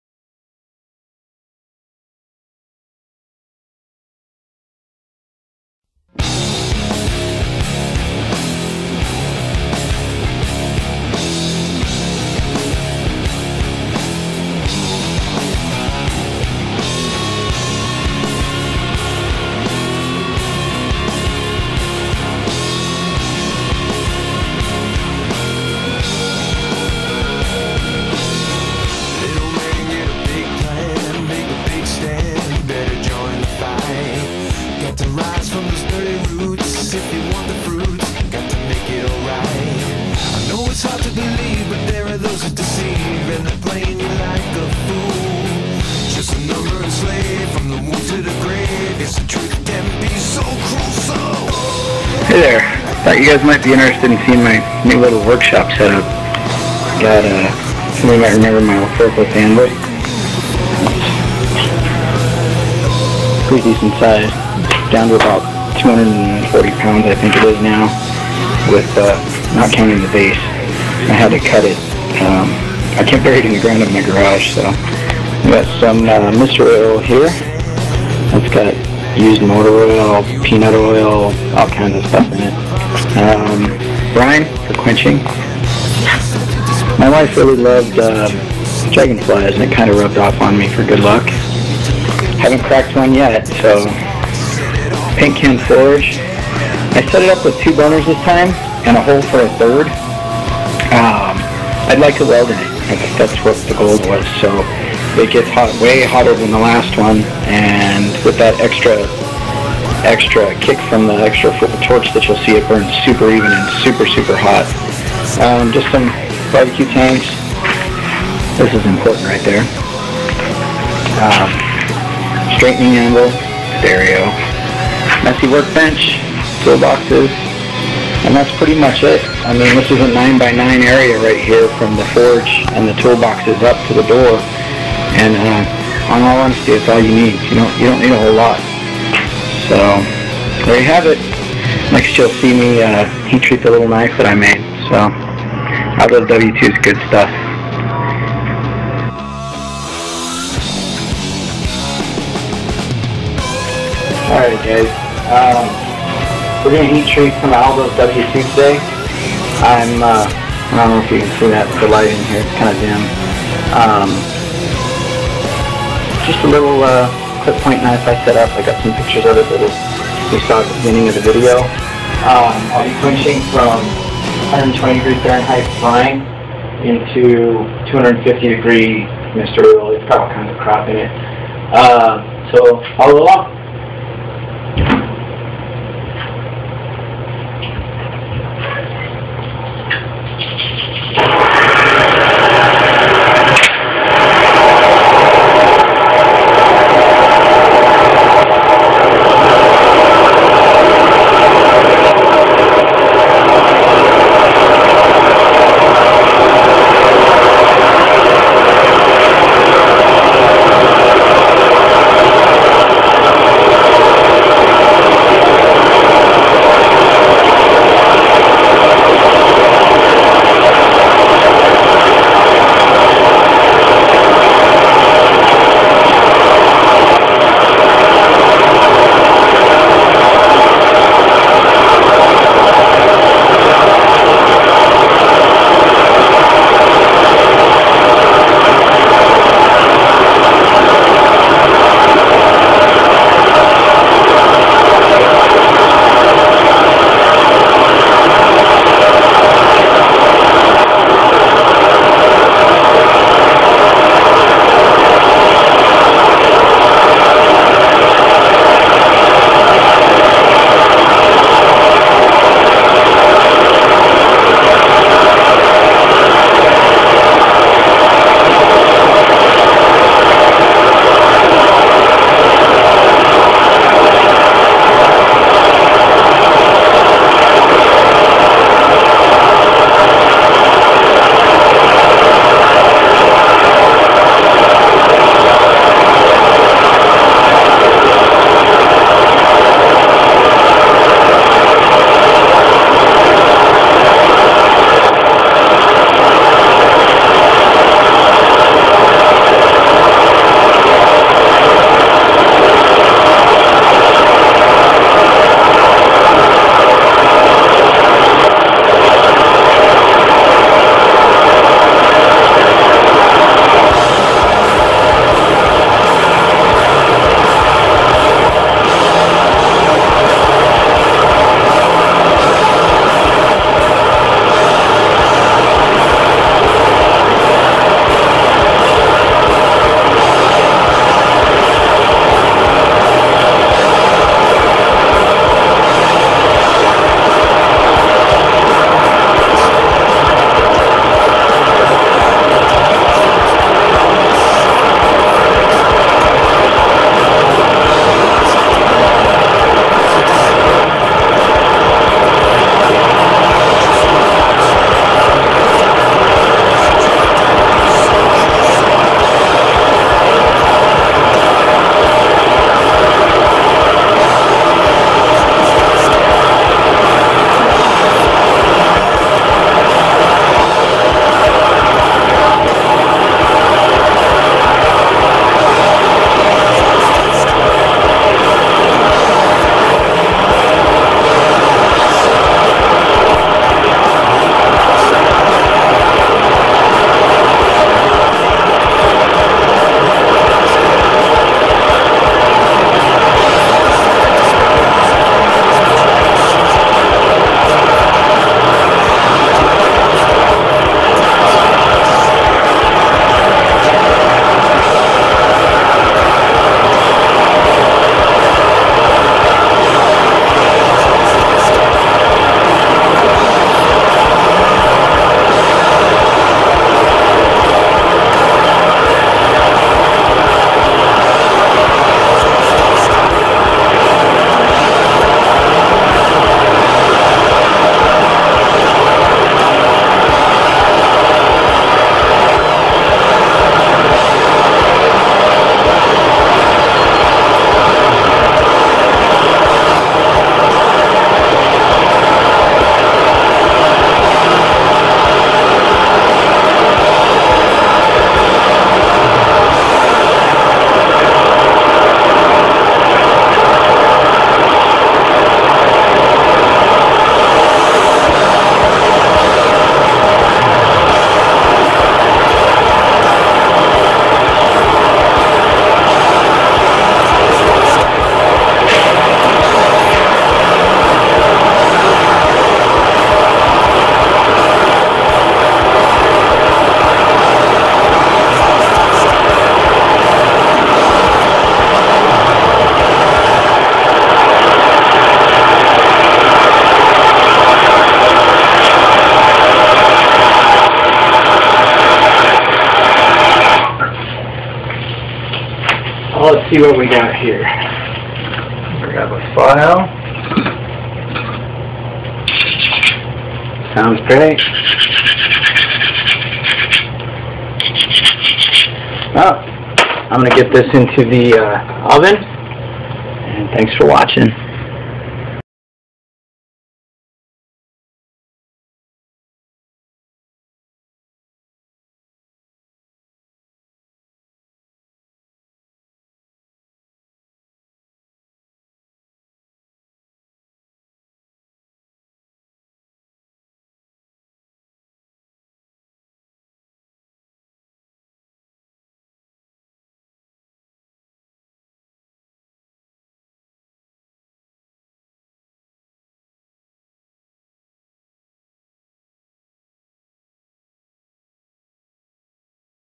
New Shop Walkthrough and W2 EDC Clip Point Heat Treat
A short video including walking through my new work area.  It's 9' x 9', and houses not only my forge and anvils, but a work bench, fan, stereo, and two rolling tool boxes.

Also included is me heat treating a W2 knife in brine.